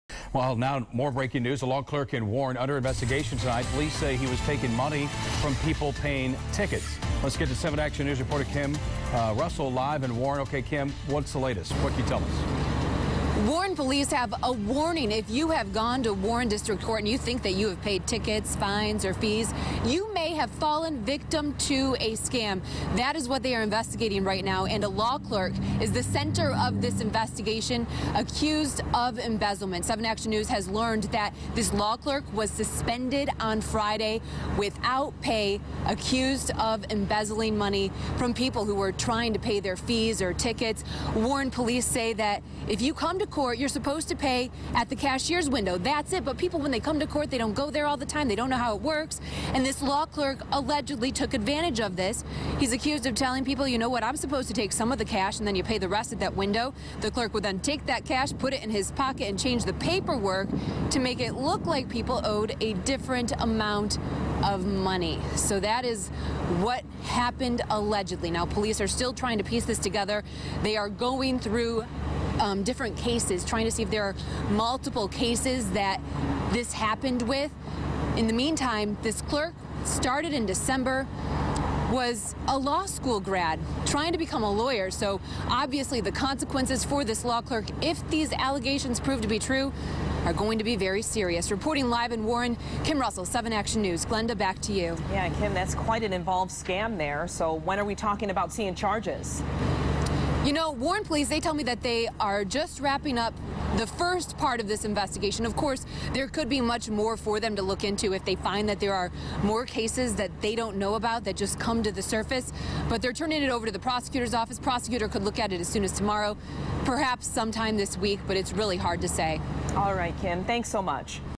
Warren law clerk accused of embezzlement
A Warren law clerk is accused of embezzling funds at the court house.